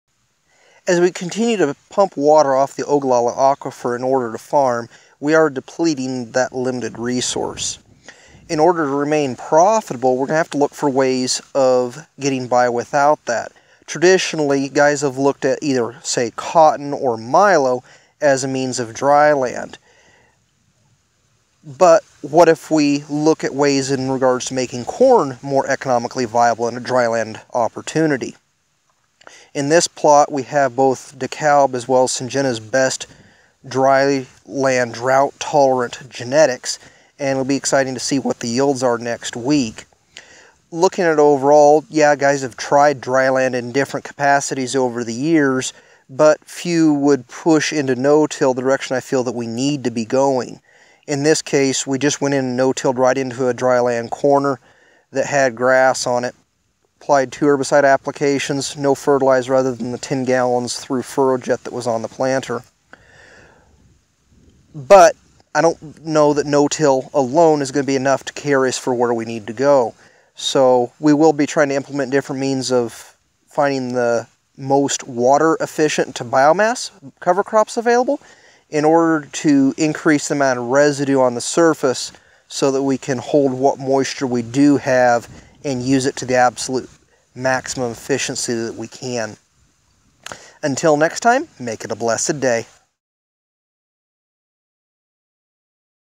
Dryland Corn Plot Overview
Join us as we talk about how we can look for way to make dry land corn economically viable and not have to resort to selling out as ones only means of survival.